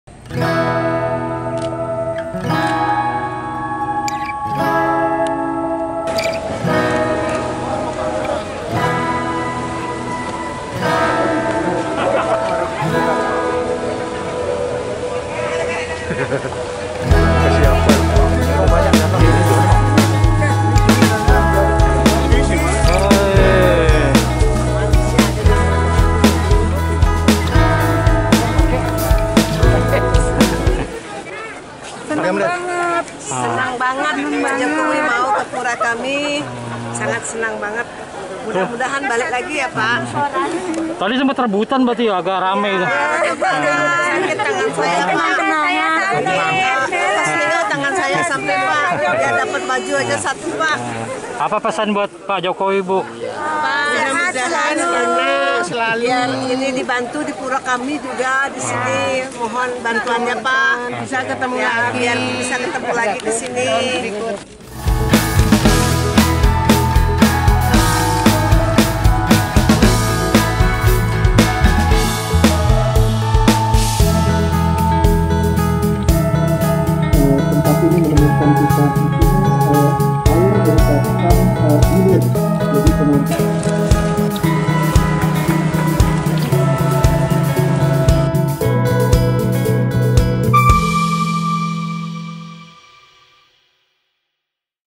Warga Berebut Kaus Gambar Jokowi saat Presiden Tinjau KTT G20 di Nusa Dua
Presiden Joko Widodo ke Bali untuk meninjau penyelanggaraan KTT G20 di Nusa Dua.

Android: inews.id/android
iPhone: inews.id/ios